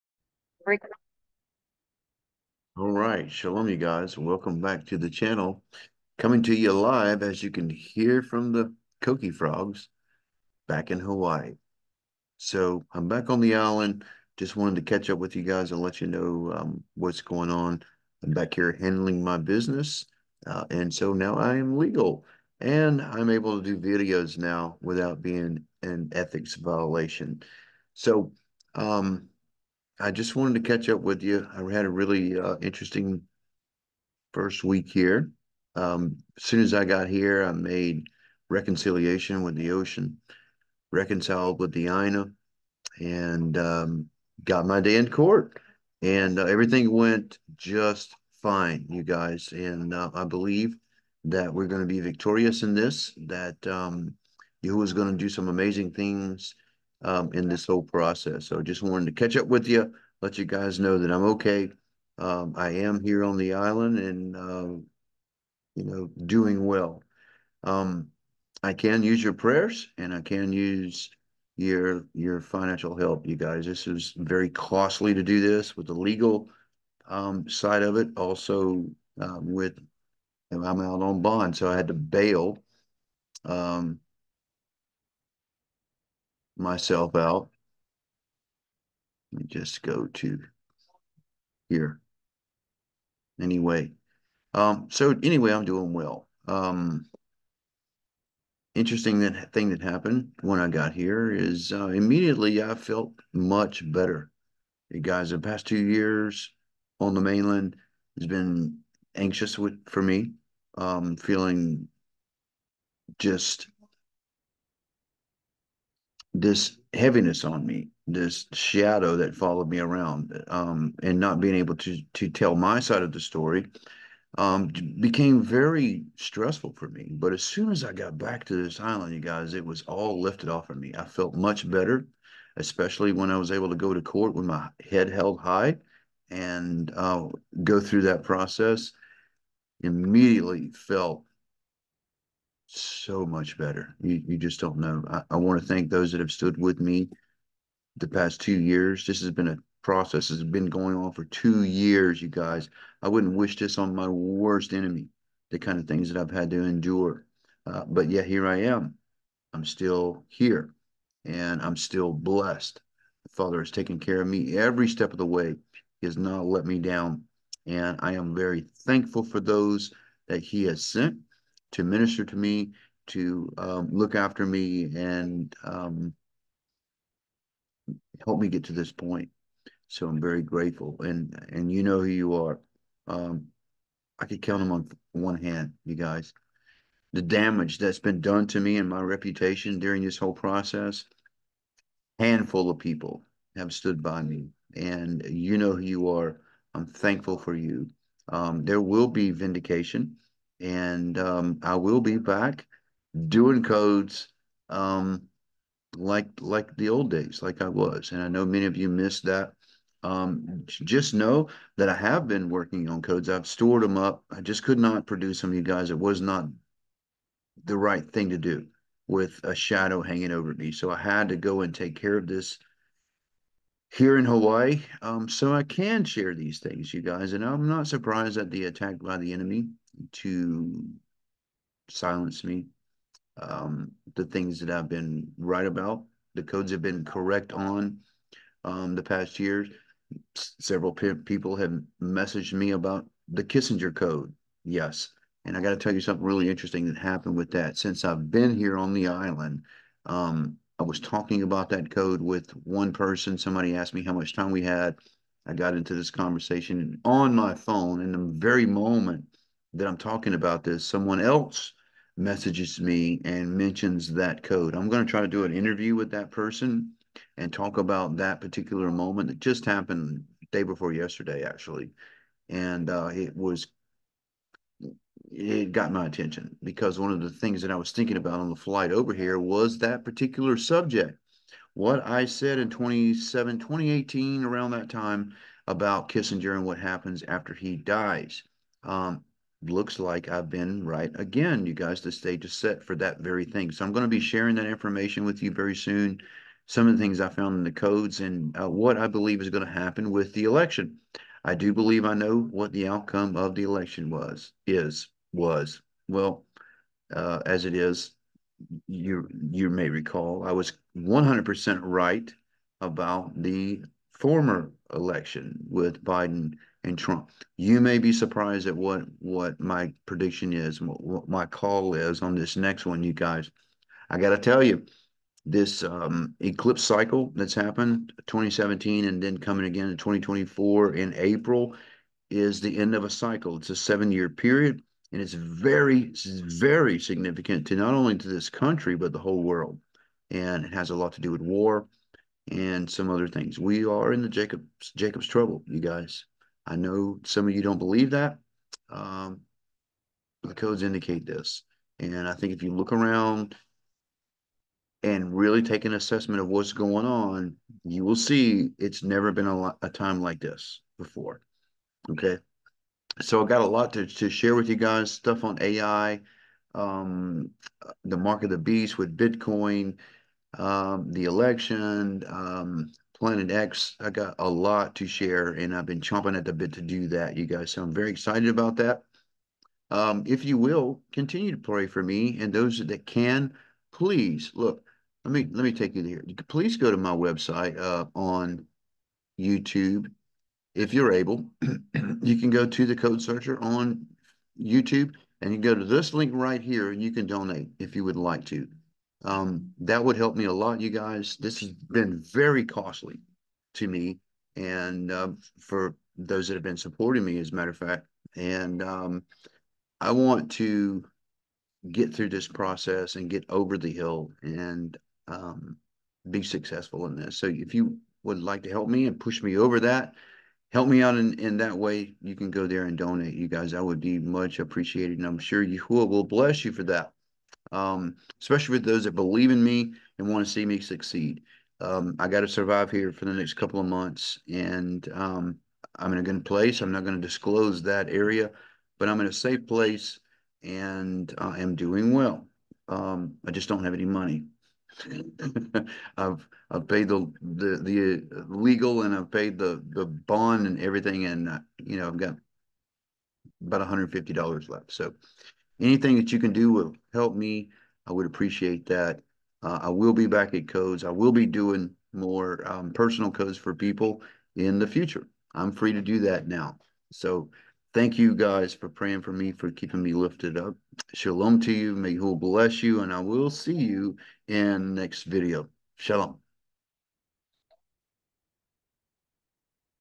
TheCodeSearcher Update- Back in HAWAII...i am well.
jonathan the CodeSearcher updates his subscribers on the situation in Hawaii.